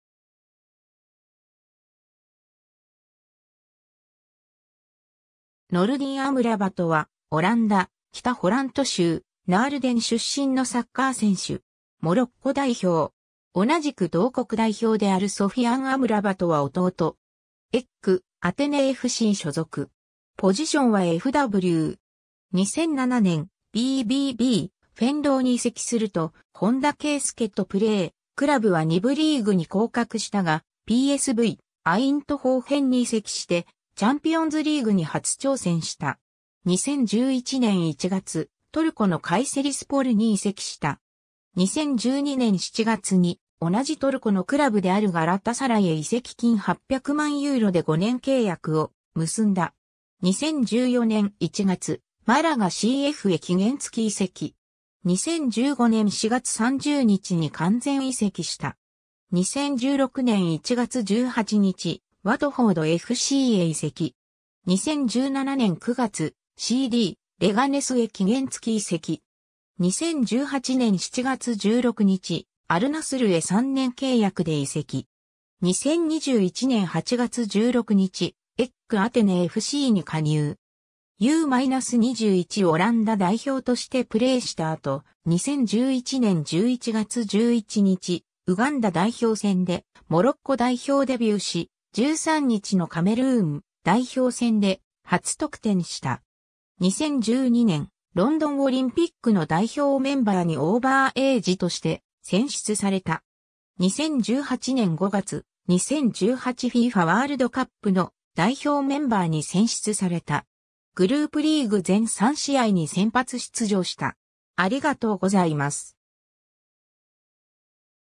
ノルディン・アムラバト
アルメレ・シティFCの選手
ノルディン・アムラバト（Nordin Amrabat アラビア語: نورالدين أمرابط‎, 1987年3月31日 - ）は、オランダ・北ホラント州ナールデン出身のサッカー選手。
モロッコ代表。
同じく同国代表であるソフィアン・アムラバトは弟。
AEKアテネFC所属。
 2007年、VVVフェンローに移籍すると本田圭佑とプレー、クラブは2部リーグに降格したが、PSVアイントホーフェンに移籍してチャンピオンズリーグに初挑戦した。
2011年1月、トルコのカイセリスポルに移籍した。
 2012年7月に同じトルコのクラブであるガラタサライへ移籍金800万ユーロで5年契約を結んだ。
 2014年1月、マラガCFへ期限付き移籍。
2015年4月30日に完全移籍した。
 2016年1月18日、ワトフォードFCへ移籍。
2017年9月、CDレガネスへ期限付き移籍。
 2018年7月16日、アル・ナスルへ3年契約で移籍。
 2021年8月16日、AEKアテネFCに加入。
 U-21オランダ代表としてプレーした後、2011年11月11日ウガンダ代表戦でモロッコ代表デビューし、13日のカメルーン代表戦で初得点した。
 2012年、ロンドンオリンピックの代表メンバーにオーバーエイジとして選出された。
 2018年5月、2018 FIFAワールドカップの代表メンバーに選出された。
グループリーグ全3試合に先発出場した。